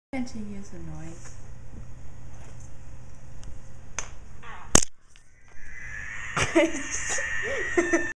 Dog punches the camera
Pepper hits the camera with her paw when I try to show her videos of angry cats